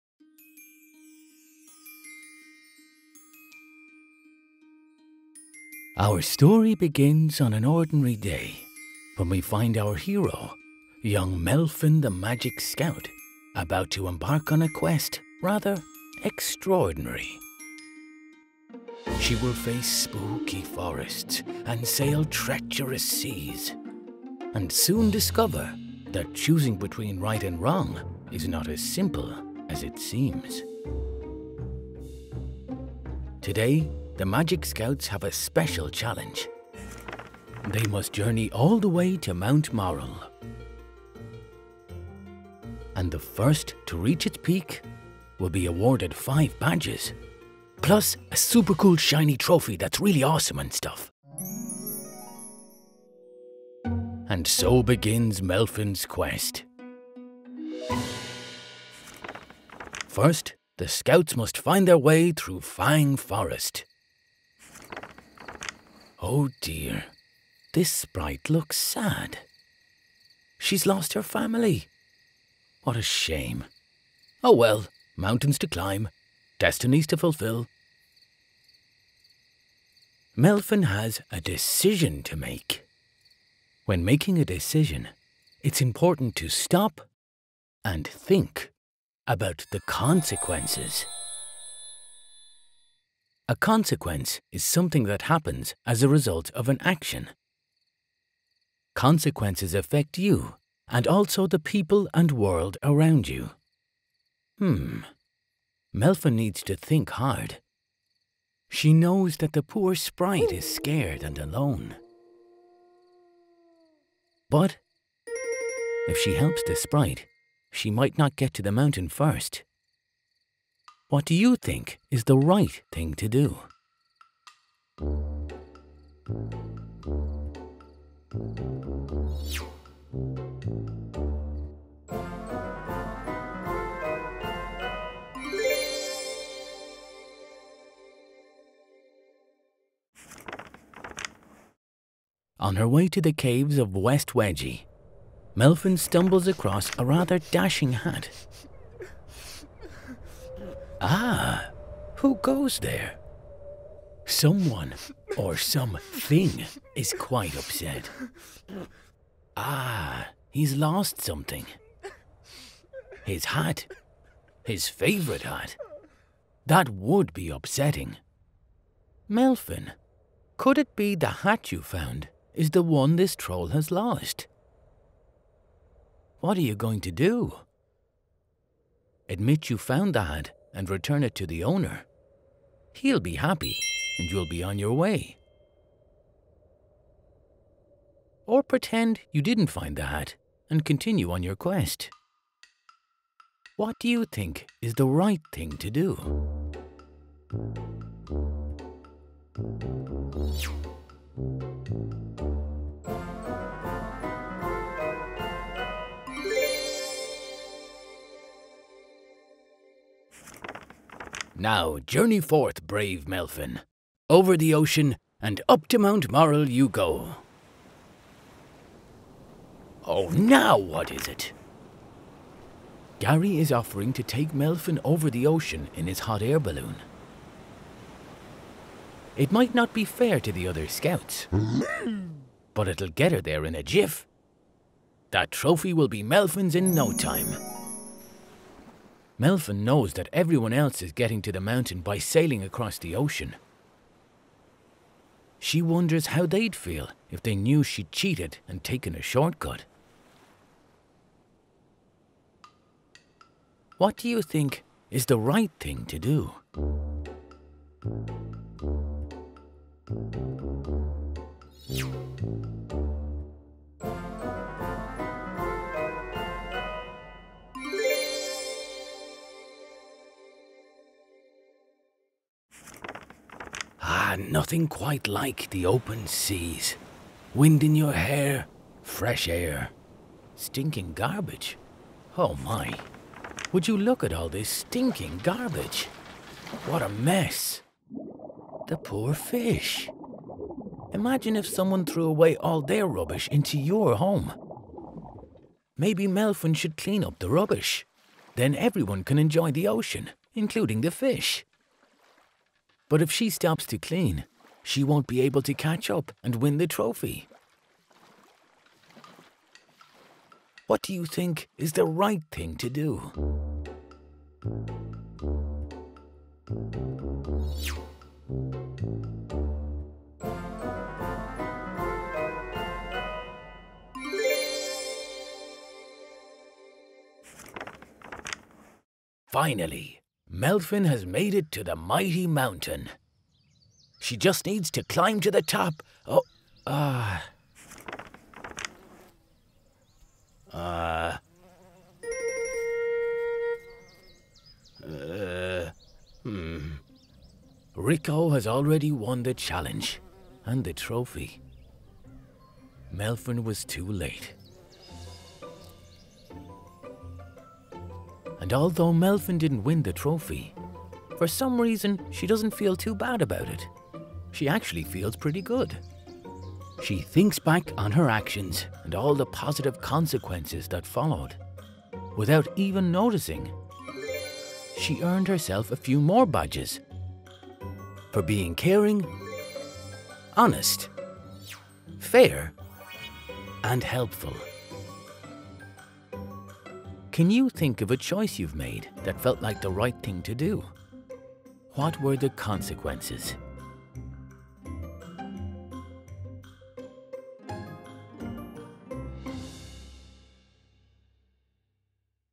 Ethics For Kids: Right And Wrong
Access lesson resources for this video + more elementary health videos for free on ClickView ✅

Melfyn the Magic Scout is on an extraordinary quest to reach Mount Moral and win the trophy! On her journey, she will encounter tricky situations that will test her ability to make the right choice.

This magical video takes students on an ethical journey to learn about right and wrong, and what consequences are. They will learn about how their choices impact others as well as themselves, and may even discover that in the end, it’s not winning a prize that counts, but how they feel inside when they do the right thing…